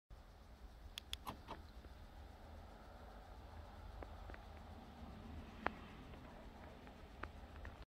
계속 보게되는 신비로운 웰컴라이트
차문을 열면 화려하게 반겨주는 헤드라이트